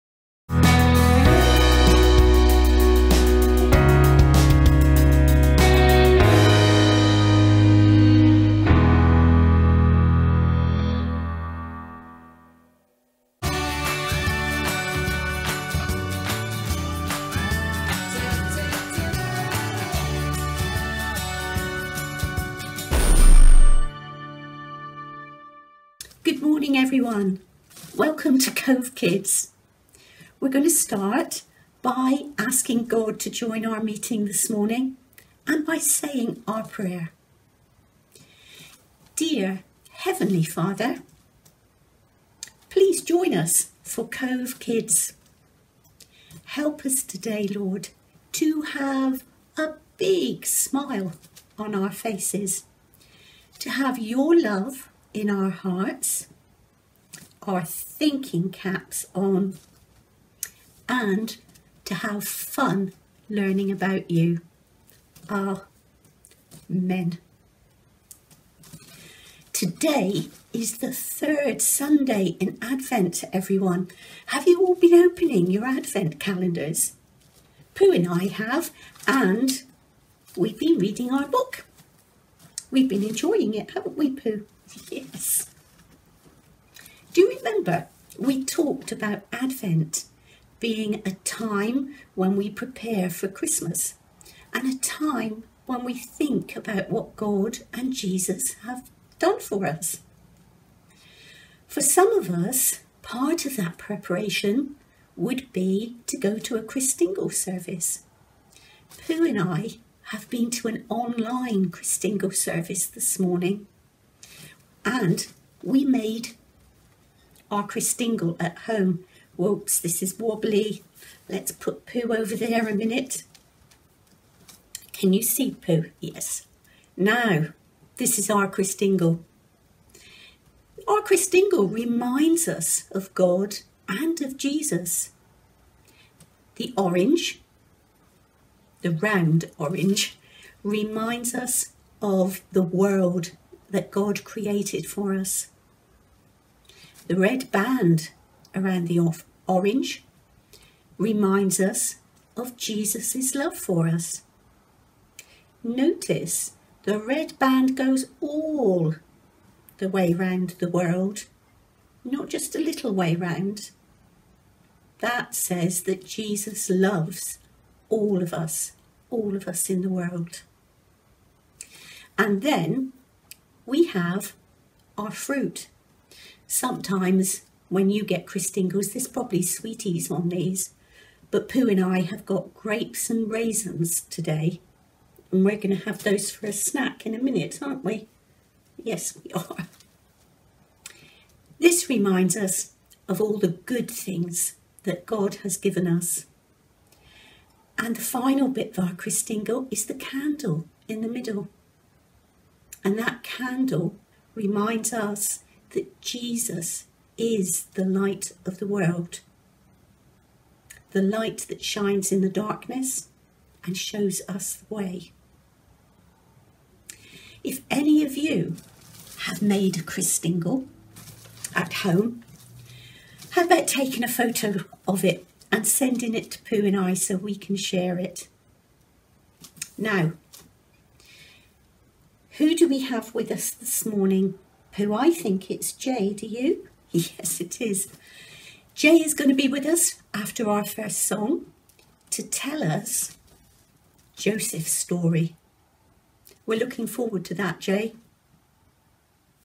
Joseph's Story
Join us for more fun at Cove Kids as we continue looking at the Christmas story today, on the third Sunday of advent with Joseph Story about how he reacted to the surprise he received. We would love you to join us from 9:45am and why not invite your friends and family to join us too by sharing this!

and Sue Bentley for the images